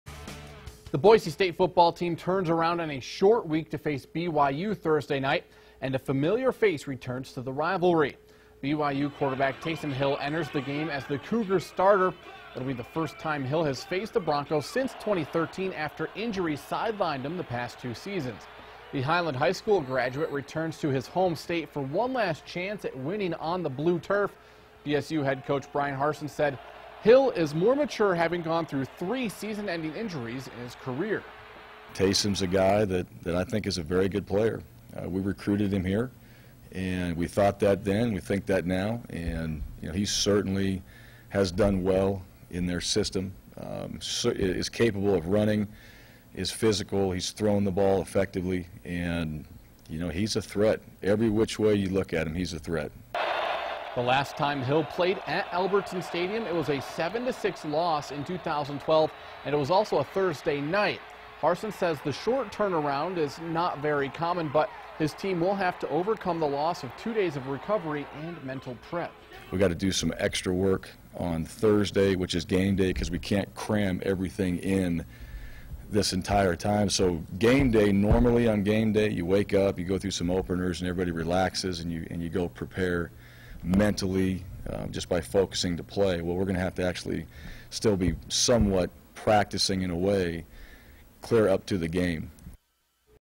Harsin: Hill is a threat
Boise State head coach Bryan Harsin speaks about BYU quarterback and Highland High School graduate Taysom Hill prior to the Broncos' game Thursday night against the Cougars.